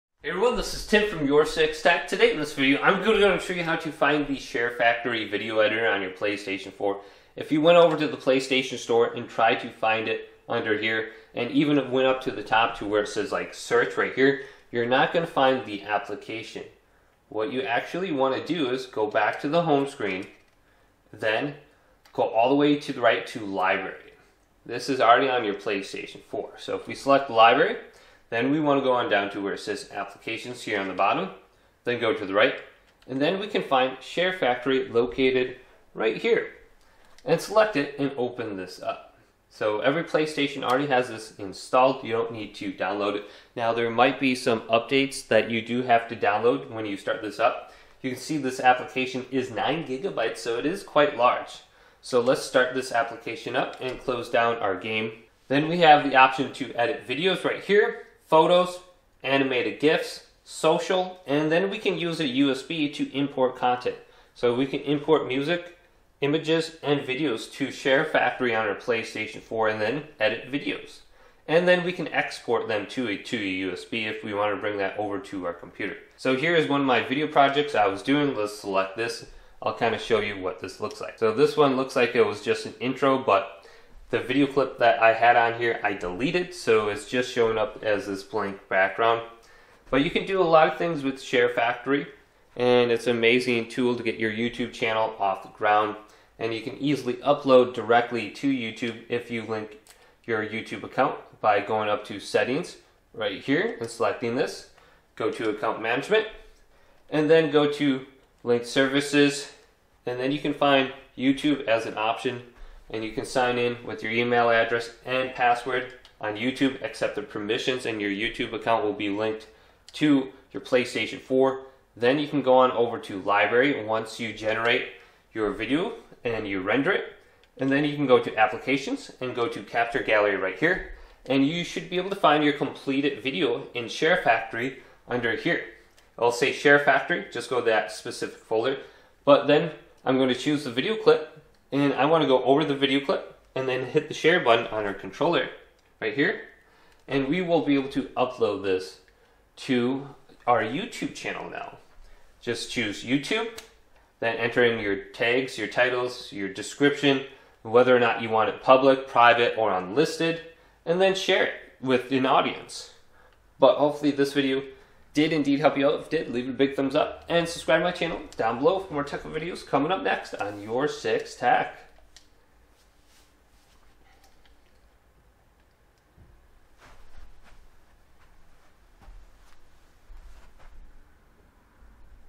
How to Get the Sharefactory Video Editor on PS4 Console (Easy Tutorial)
To get the Sharefactory video editor on your PS4 console, you can follow these steps:

Turn on your PS4: Ensure that your PS4 console is powered on and connected to the internet.

Access the PlayStation Store: From the PS4's home screen, navigate to the PlayStation Store. You can find the PlayStation Store icon in the main menu.

Open the Apps section: Once you're in the PlayStation Store, scroll down and select the "Apps" category. This section contains various applications available for download.

Find the Sharefactory app: In the Apps section, you can search for "Sharefactory" using the search function or browse the available apps. You can use the D-pad or the thumbsticks on your controller to navigate and select items.

Download and install Sharefactory: When you find the Sharefactory app, select it, and choose the "Download" or "Add to Cart" option. Sharefactory is a free app, so you should be able to directly download it without any purchase.

Wait for the installation to complete: After selecting the download option, the PS4 will start downloading and installing Sharefactory on your console. The progress of the installation will be displayed.

Access Sharefactory: Once the installation is complete, you can find the Sharefactory app in the PS4's main menu or library. Look for the Sharefactory icon and select it to launch the app.

Start editing videos: After launching Sharefactory, you can use it to edit and create videos on your PS4. The app provides various features, such as video trimming, adding text and stickers, applying filters and transitions, and including audio tracks. You can import videos captured with the PS4's Share button or copy videos from a USB drive to edit in Sharefactory.